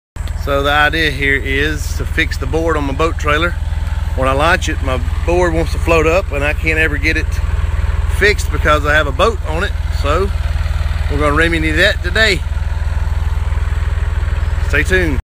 2003 Honda Rancher and 1976 MF135 fix a 2001 Polarkraft boat trailer.
For PistonHonda. Thanks.